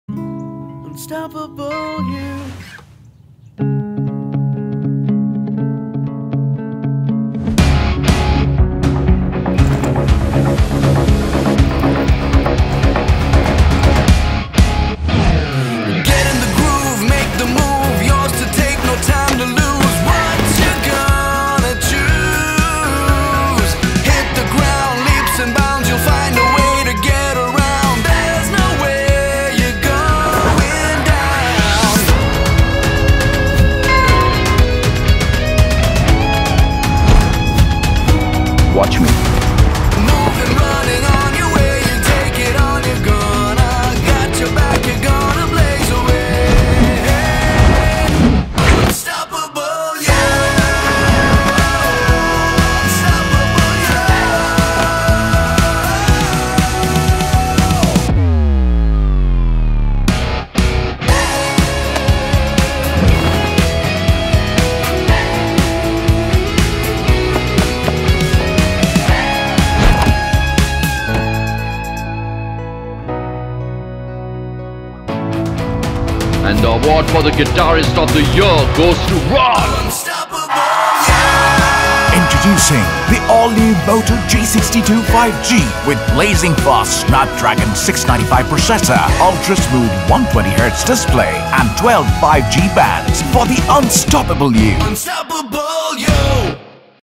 Introducing the all new motog62 5G! Sale starts 19th August on Flipkart.
Say hello to all-new motog62 for the UnstoppableYou. Experience unstoppable speed of True 5G connectivity with 12 5G Bands, blazing fast Snapdragon™ 695G with 6/8GB RAM, smooth 120Hz FHD+ 6.5” Display with powerful Dolby Atmos® stereo speakers & more starting at just ₹16,249*. Sale starts 19th August on @Flipkart!